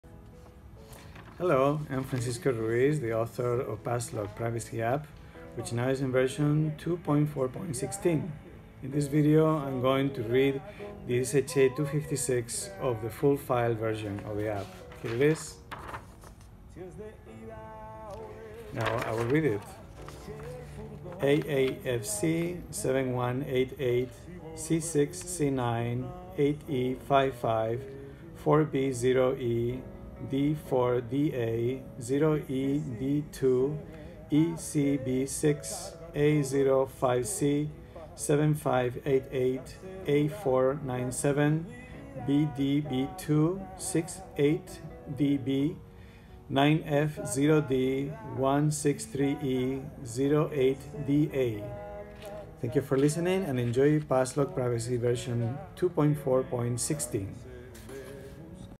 F. Ruiz reads the SHA256 of PassLok 2.4.16
PassLok 2.4.16 is focused mainly on mobile devices. Encrypted content now get copied automatically when the SMS button is tapped. Rich text is also available, including the ability to add pictures and files.

SHA256 for all sources (single file):
aafc-7188-c6c9-8e55-4b0e-d4da-0ed2-ecb6-a05c-7588-a497-bdb2-68db-9f0d-163e-08da